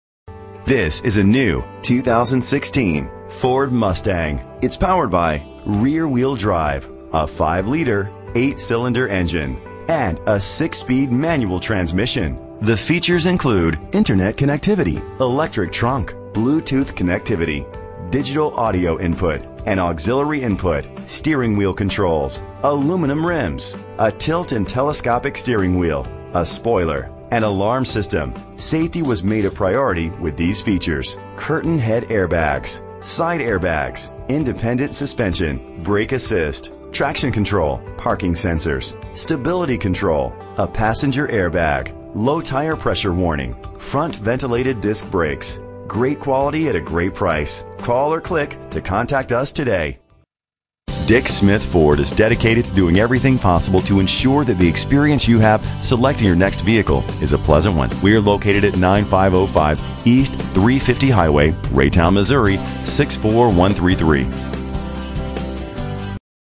2016 Ford Mustang - Raytown MO
Phone:
Year: 2016
Make: Ford
Model: Mustang
Trim: GT
Engine: 5.0 liter 8 cylinder 32 valve
Transmission: 6-Speed Manual
Color: Race Red
Mileage: 2
Address:
9505 East Hwy 350